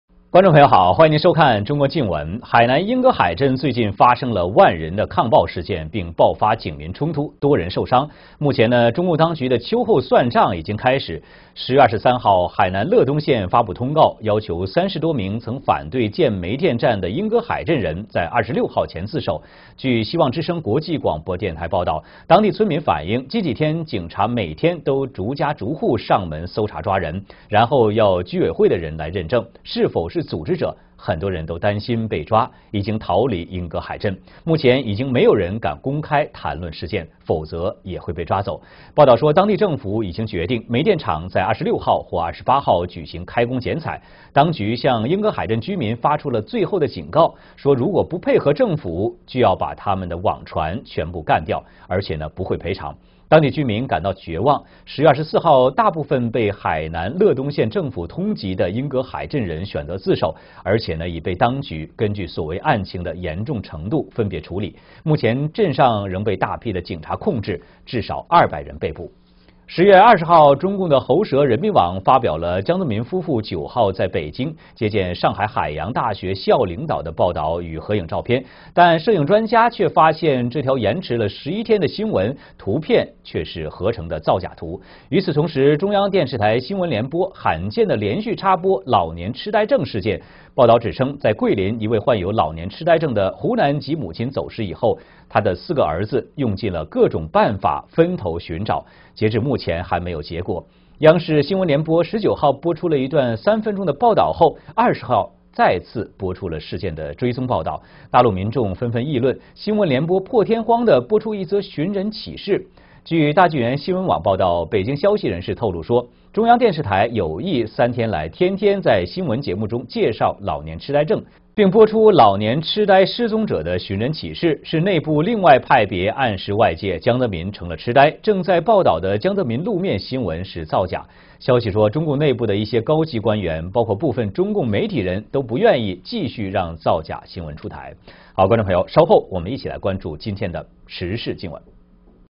Police Arrest Mass Protesters in Hainan
Recently, thousands of people held protests
in Yinggehai township, Hainan Province.
Protesters clashed with police, and many were injured.

Now the Chinese Communist Party (CCP)
has started it's punishment on protesters.

On Oct.23, Ledong County,
in Hainan Province, released a notice.
It requested protesters, who had been against the launch
of a local coal power plant, to surrender before October 26.

Sound of Hope Radio International reported local
villagers saying that
everyday, police are searching for protesters
door to door, and taking villagers away.
Police asked the residential committee to check
if detainees are organizers of the protest.
Many people are worried about being
detained, and have had to leave home.
Now no one dares to mention the incident,
otherwise, they will be arrested.

The report said that local authorities
had decided to go ahead with plant project.
They will hold a ribbon-cutting
ceremony on October 26 or 28.
The authorities also warned residents that
if they don't cooperate, their boats will be completely destroyed without compensation.

The villagers feel hopeless. On October 24, most villagers,
who were on the police wanted list, surrendered.
Later, those villagers received punishment
in accordance with their "crimes".

Now, many police are controlling the town,
with at least 200 people having been arrested.

Sources Suggest CCTV Broadcast on Alzheimer's Disease is Hint About Jiang Zemin

On October 20, the CCP state controlled People's Daily
published a photo and news about Jiang Zemin and his wife.
The report was about their visit to Shanghai
Ocean University in Beijing on October 9.
However, photographers noticed that the news
was not only 11 days late, but the photo was fake.

Meanwhile, China Central Television (CCTV)
consecutively broadcasted stories on Alzheimer's disease.
It said that in Guilin, after a mother with
"Alzheimer's disease" has gone missing.
Her four sons have tried hard
to search for her, but with no result.
CCTV broadcasted a 3 minute news report on October 19,
and on October 20, it reported follow up news.
People in mainland China noted that CCTV
has unusually reported news of a missing person.

The Epoch Times reported that a Beijing insider revealed
that CCTV has introduced the Alzheimer's disease program consecutively for three days,
In addition, it has reported news of a missing
mother who suffers with Alzheimer's disease.
This is all to hint that Jiang Zemin has Alzheimer's disease.
News on Jiang Zemin that has appeared in public is fake.

Sources said that some senior leaders in the CCP, including
some media persons, are unwilling to publish this fake news.